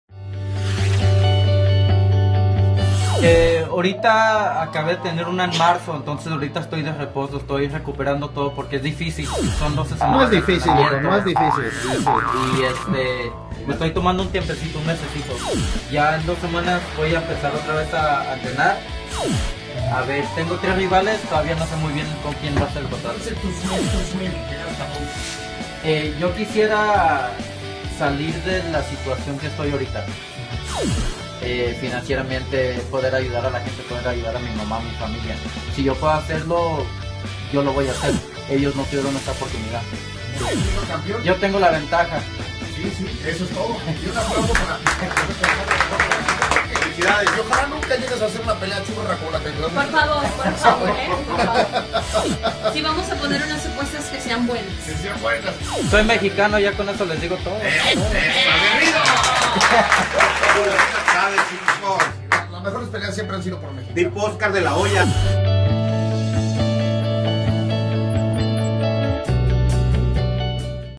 Sergio El Keko Aguirre
Entrevista con la radio La Raza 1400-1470AM en Minneapolis. Ablando sobre mi carrera del boxeo.